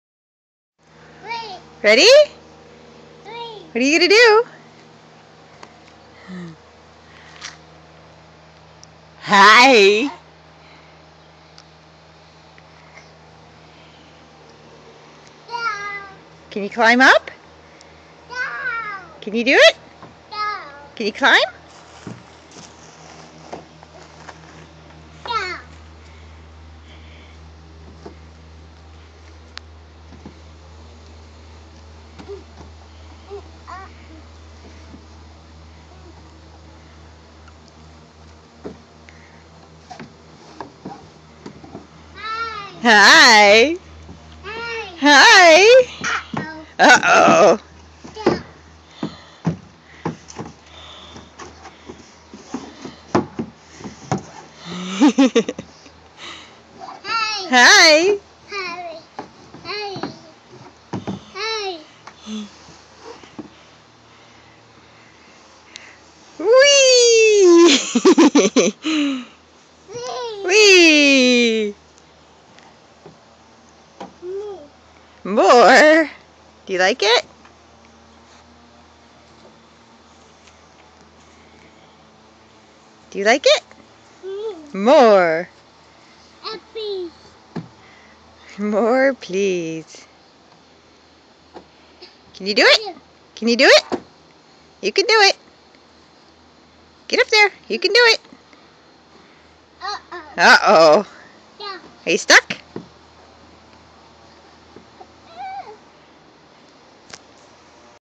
New playground!
Hobbit playing on his brand used playground :-)